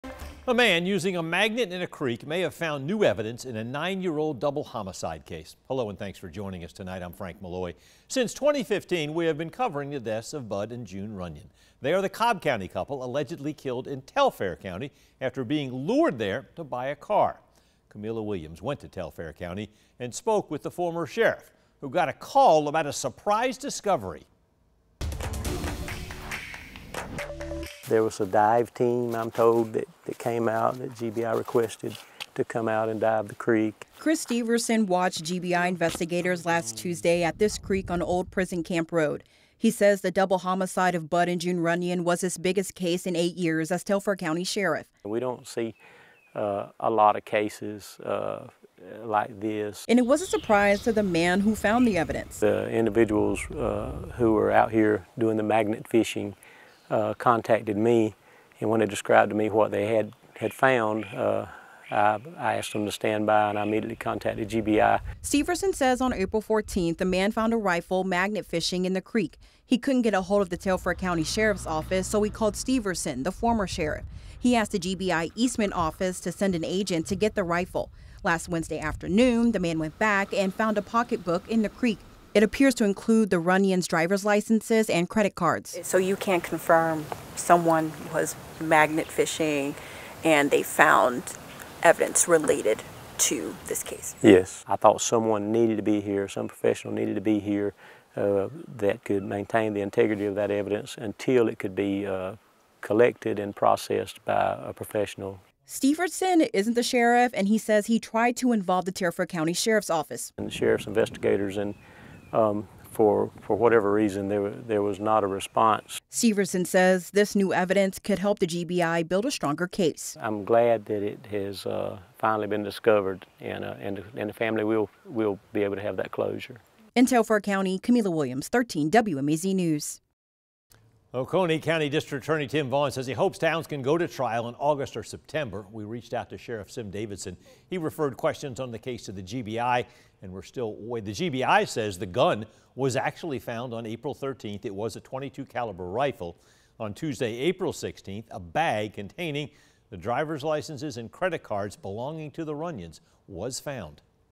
Possible evidence found in Telfair County double murder
A man was magnet fishing in a creek on Old Prison Road and found a gun and pocketbook.